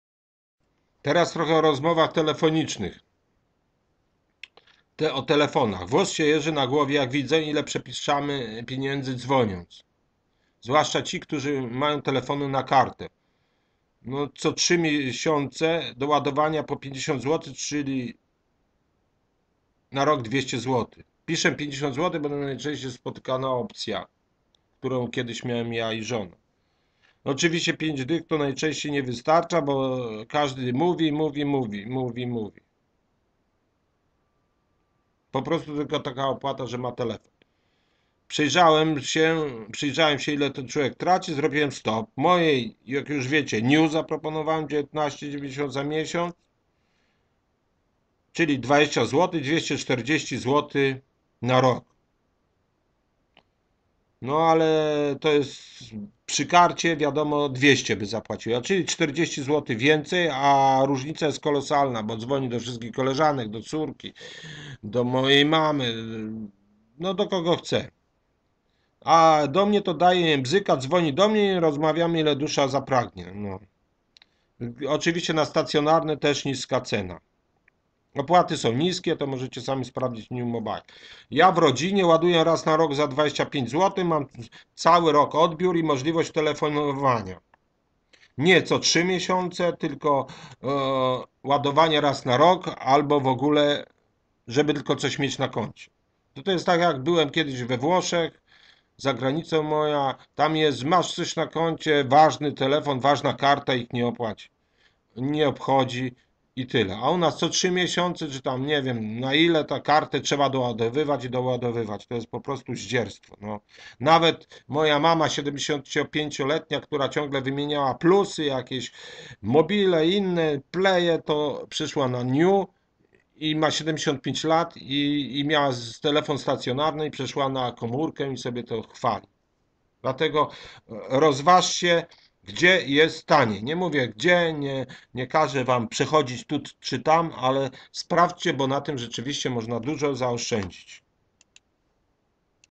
Opłaty za telefon na kartę
Jest to dźwiękowa wersja bloga napoletatu.blog.pl
Co zrobić, by rozsądnie płacić za telefon